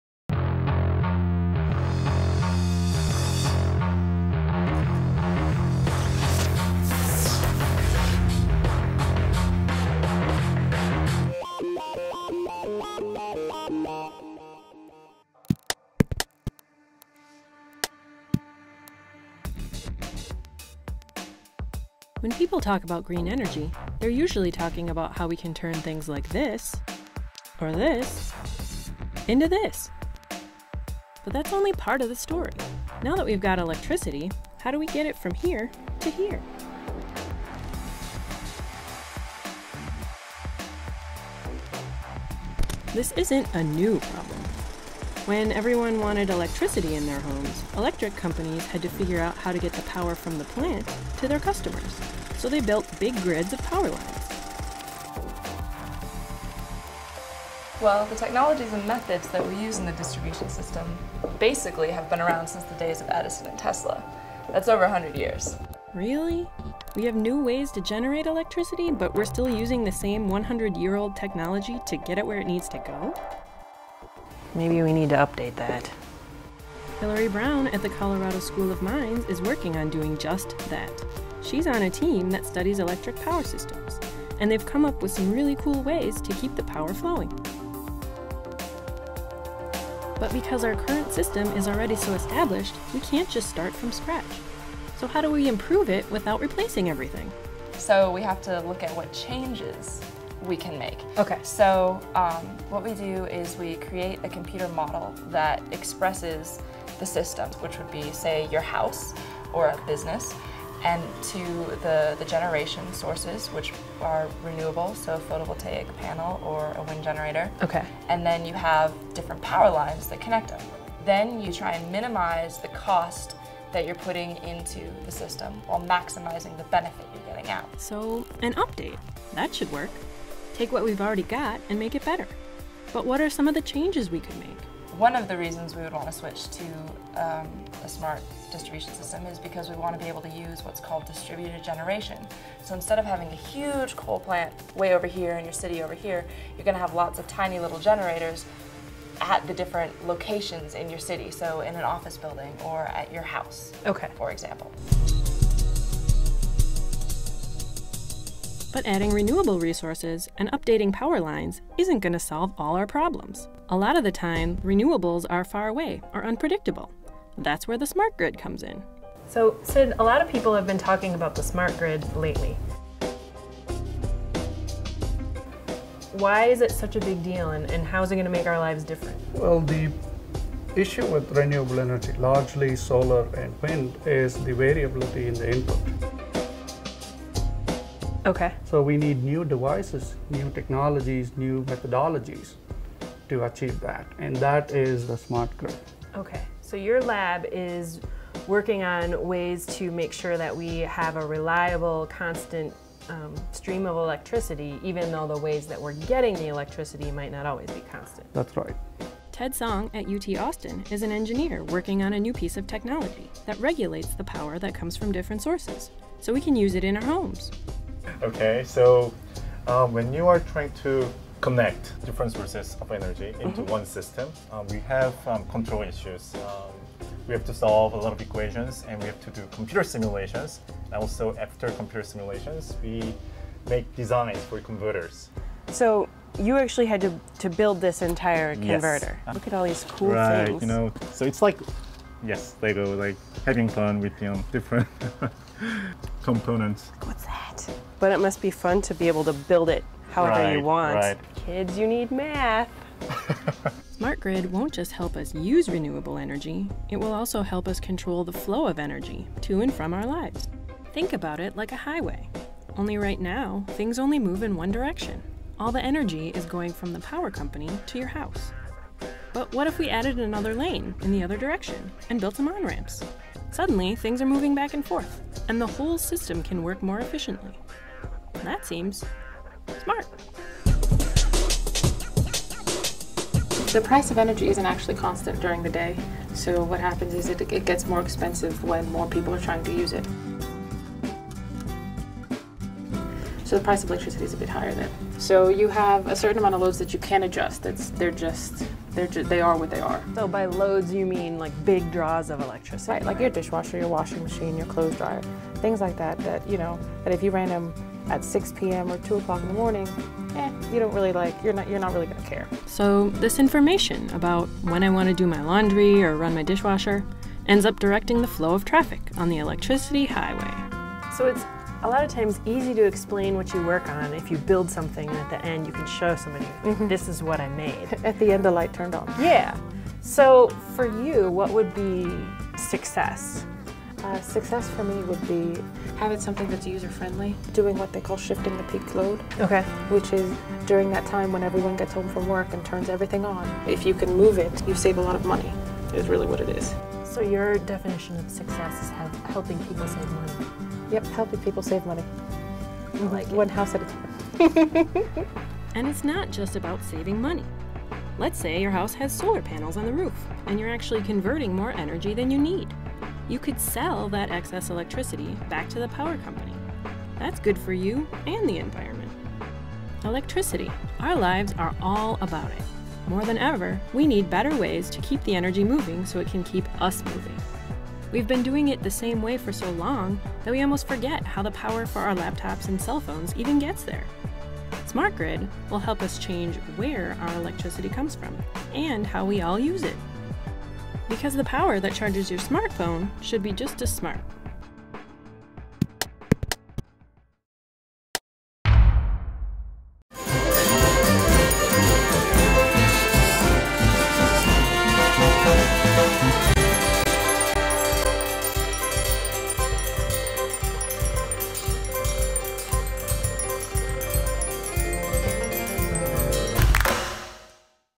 Green Revolution Series: Smart Grid
Scientist Lisa Van Pay meets with electric power experts to learn about the smart grid concept.